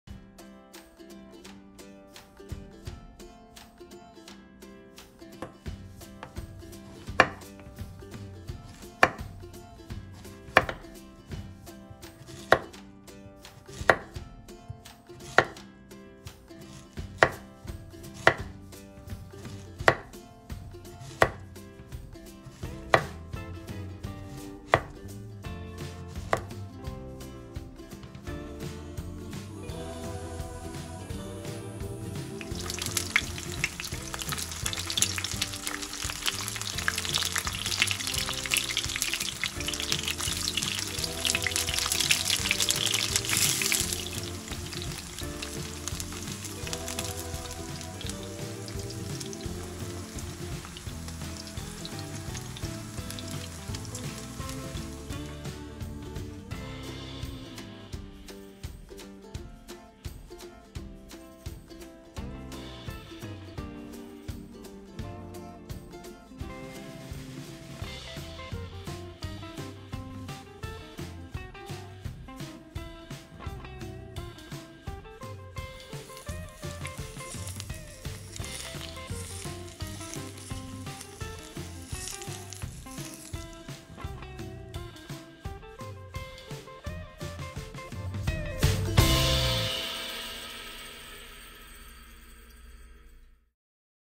밀가루, 계란 없는🈚️! 누드 애호박전 만들기/How to make Grilled Zucchini
1.애호박을 적당한 두께로 썰어주세요
2.프라이팬에 아보카도 오일이나 포도씨유처럼 발열점이 높은 오일을 넉넉히 둘러주세요
3.호박을 부쳐주세요
4.접시에 담고
5.간장 1큰술을 살짝 위에 뿌려주세요
6.남은 오일을 살짝 부어주면 완성!